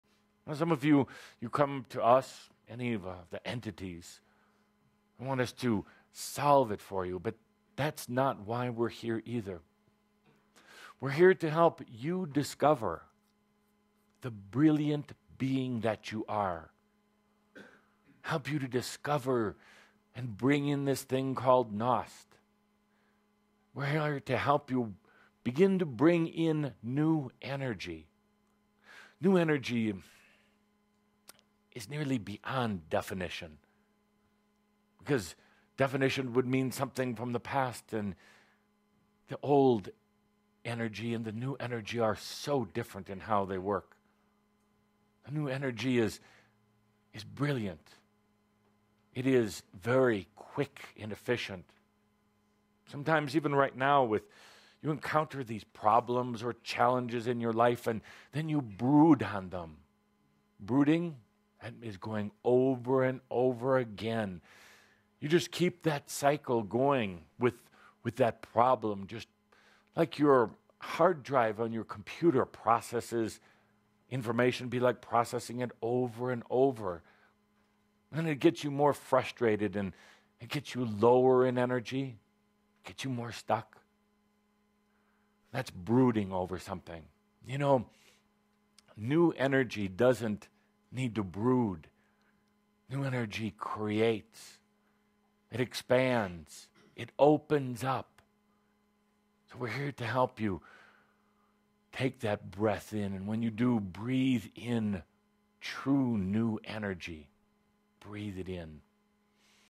1 - Kuthumi - Brooding on (self created) 'problems'
Some of you, you come to us – any of the entities – and want us to solve it for you. But that’s not why we’re here either. We’re here to help you discover the brilliant being that you are, help you to discover and bring in this thing called gnost. We’re here to help you begin to bring in New Energy.

New Energy is nearly beyond definition, because definition would mean something from the past, and the Old Energy and the New Energy are so different in how they work. The New Energy is brilliant. It is very quick and efficient.

Sometimes even right now you encounter these problems or challenges in your life and then you brood on them. Brooding – that is going over and over again. You just keep that cycle going with that problem, just like your hard drive on your computer processes information. It would be like processing it over and over. Then it gets you more frustrated and it gets you lower in energy, gets you more stuck. That’s brooding over something. You know, New Energy doesn’t need to brood. New Energy creates. It expands. It opens up. So we’re here to help you take that breath in, and when you do, breathe in true New Energy. Breathe it in.